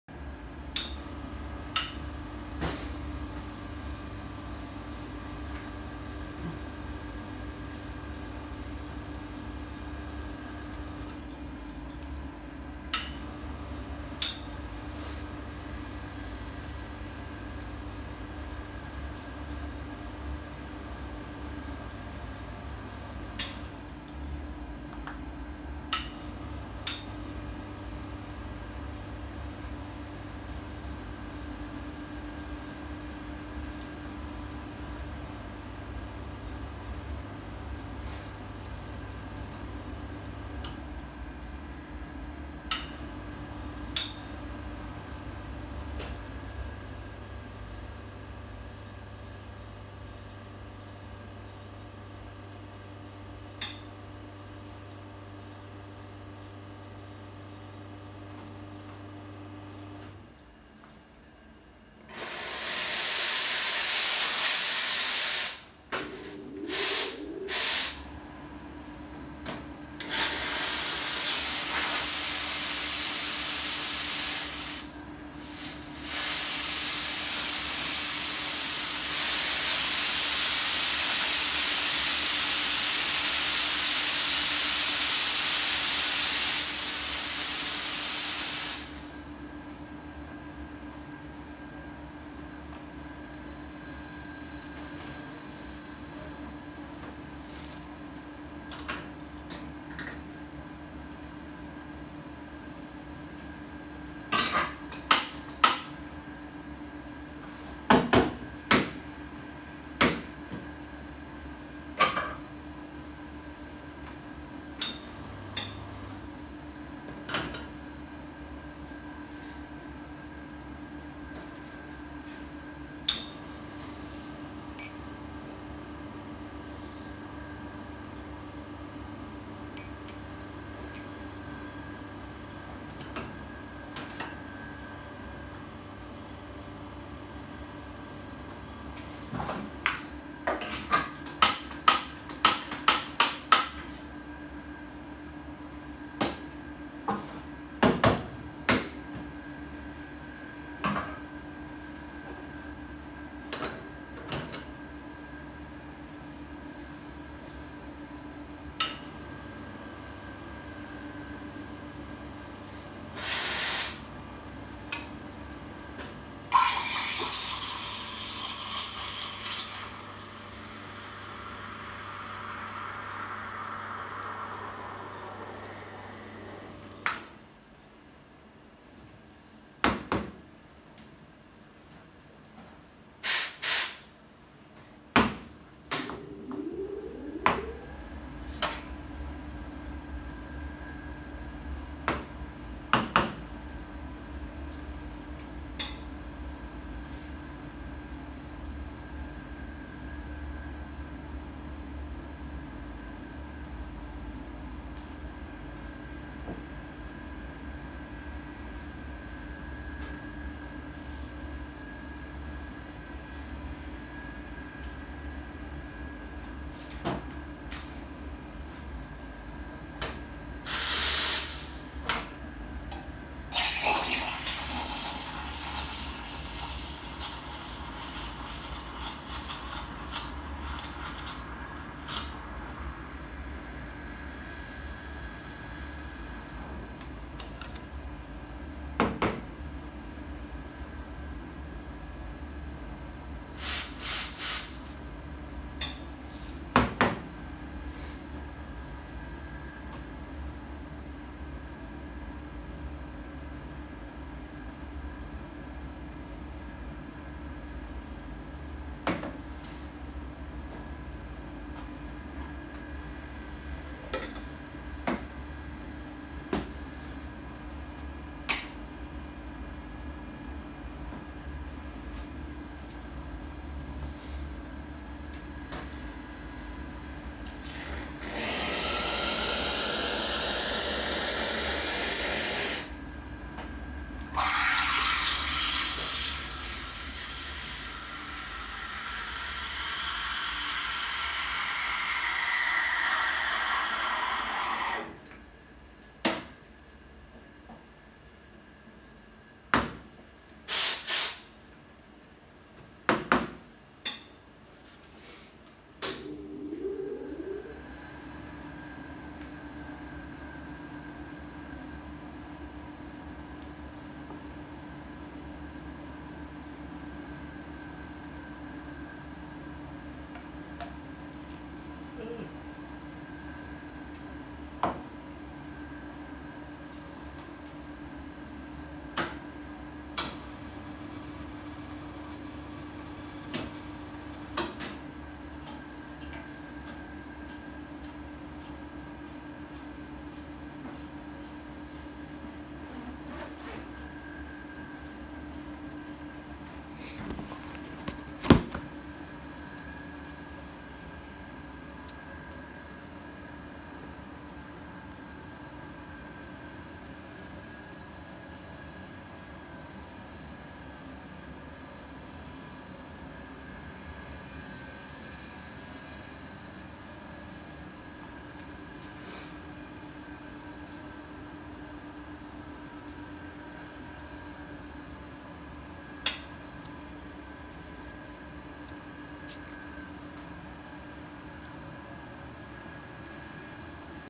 Iberital lanna refurbished espresso machine
This video demonstrates the workings of a 2 group Iberital espresso machine